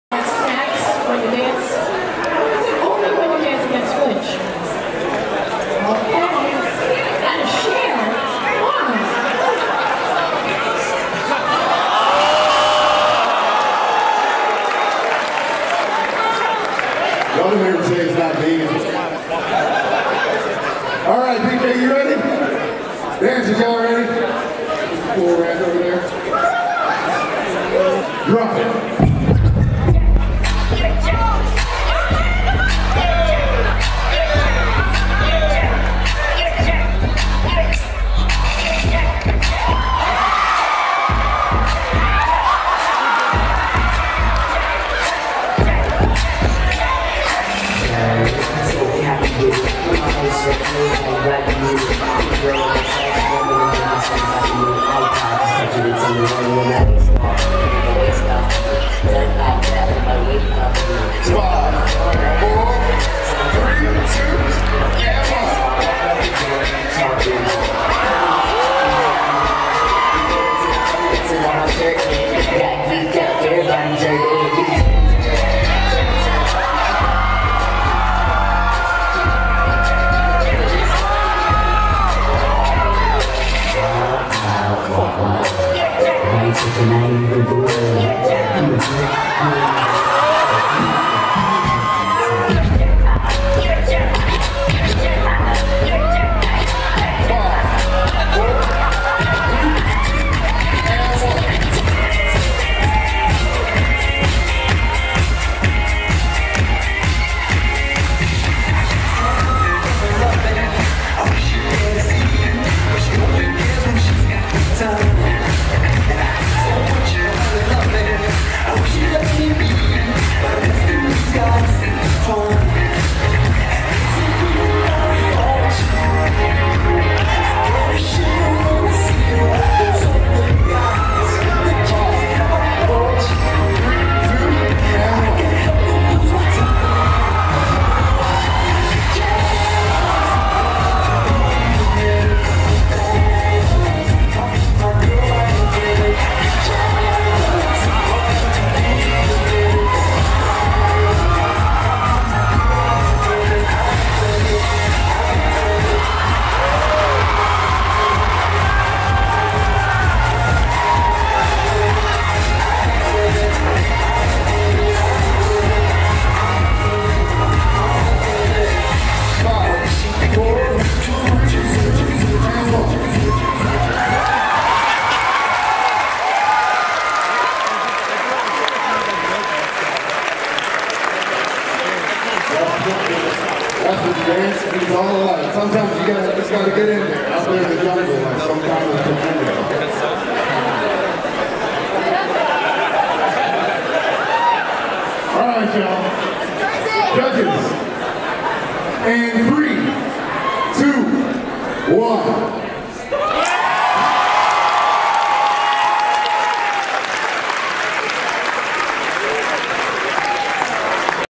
MFF 2018 Floor Wars: Flinch vs Bemani
Singles/Nonsuit Round 1

Anybody getting BLFC Dance Battle Royale flashbacks here?

Thanks to Shiro Kuma for letting me use the audio from his recordings to touch up on my videos. You'd think I've learned not to place myself so near to the speakers by now XD